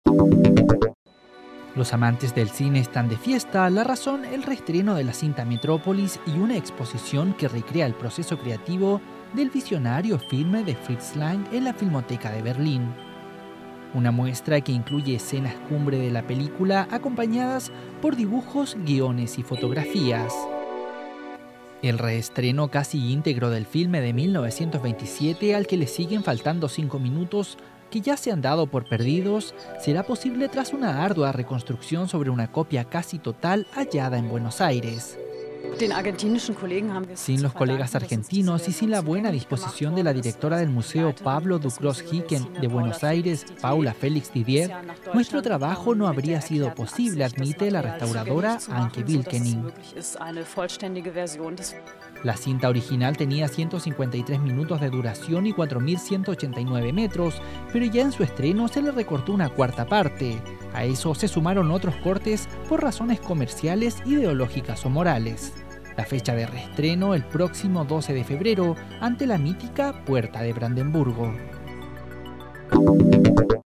Muestra recrea "Metrópolis" previo a su reestreno
Berlín (Alemania), 20 ene (EFE), (Imagen: Ingrid Haack).- La Filmoteca de Berlín recrea desde hoy, con una exposición, el proceso creativo de "Metrópolis", el visionario filme de Fritz Lang estrenado en 1927 y que tendrá su reestreno en febrero ante la Puerta de Brandeburgo, tras una ardua reconstrucción sobre una copia casi íntegra hallada en Buenos Aires.